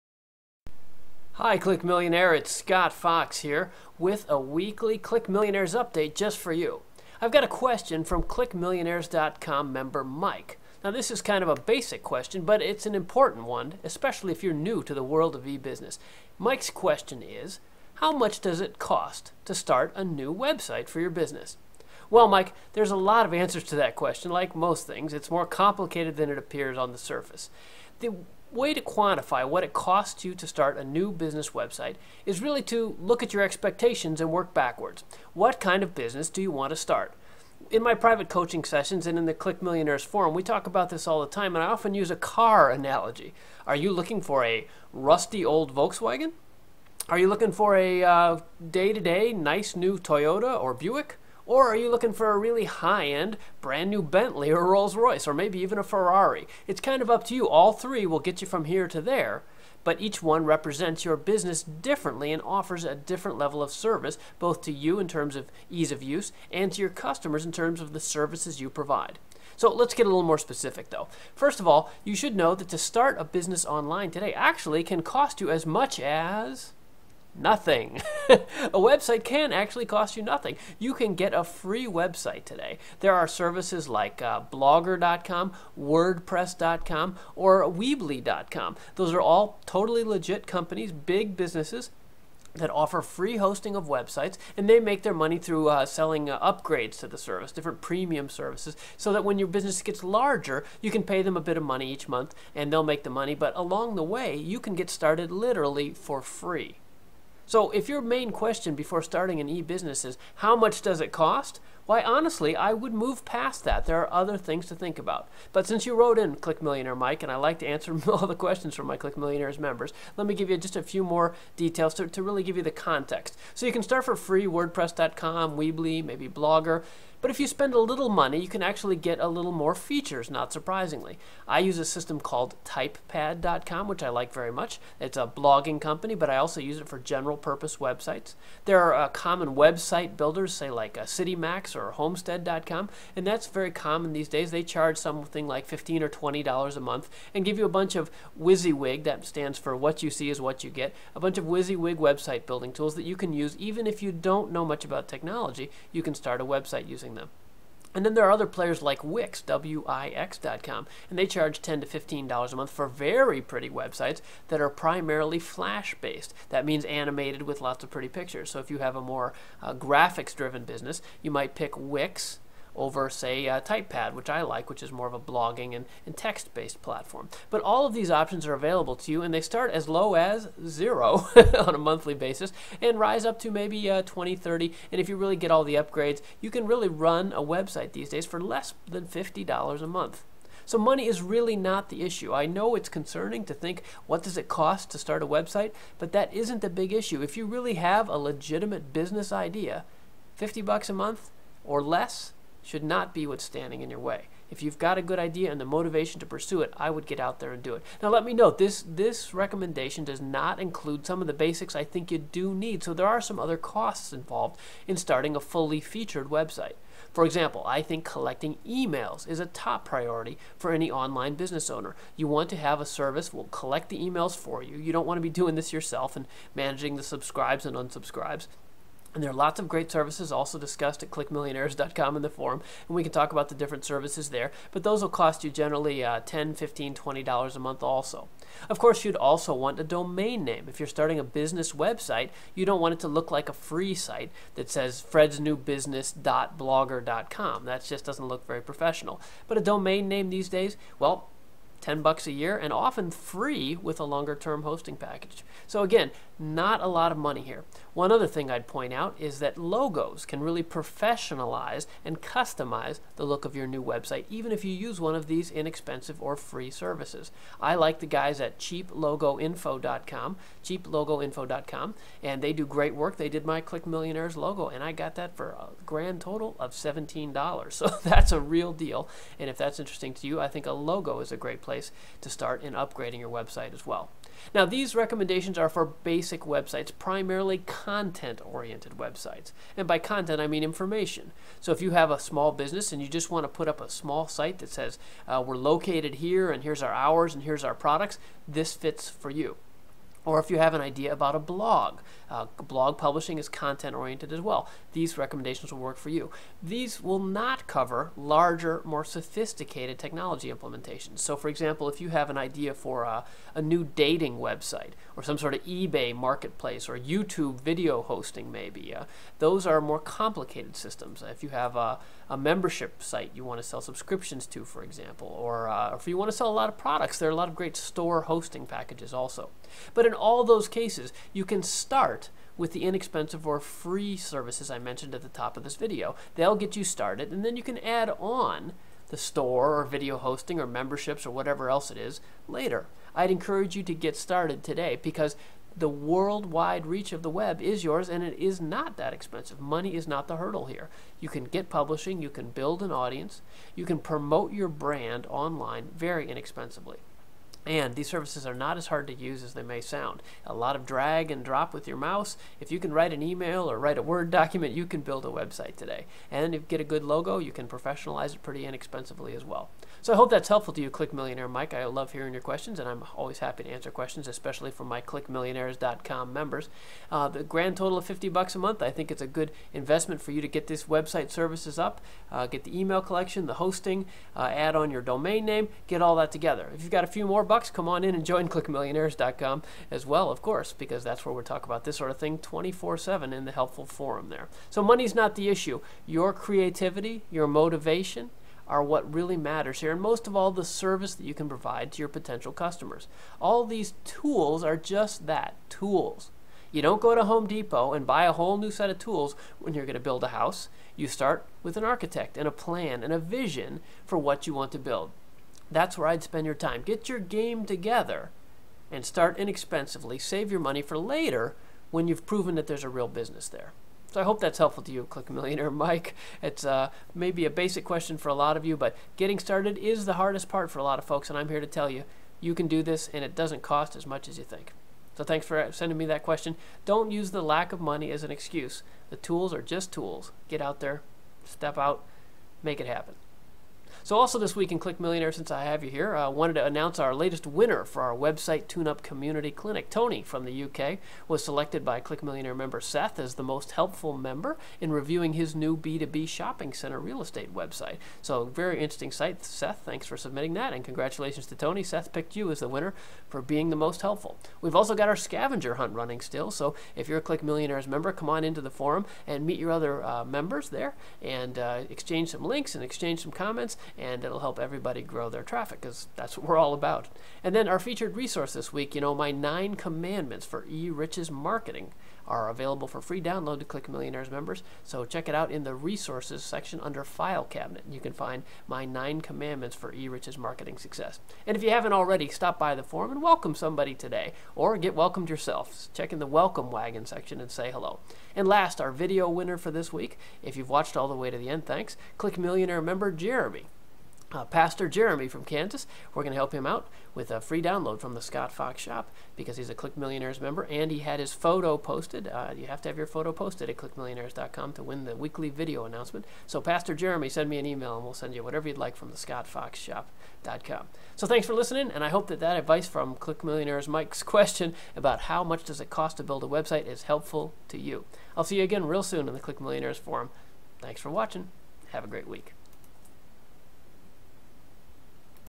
How Much Does it Cost to Build a Web Site?
- How much does it cost to build a web site?  Scott Fox answers this ClickMillionaires.com member question with helpful info from free web site hosting services Wordpress, Weebly, and Blogger, plus paid web site builders like Typepad, Wix, Citymax, and more web site building hosting services you can use to start a new web site business online.
for more expert online marketing advice and personal coaching from best-selling author and e-commerce expert, Scott Fox.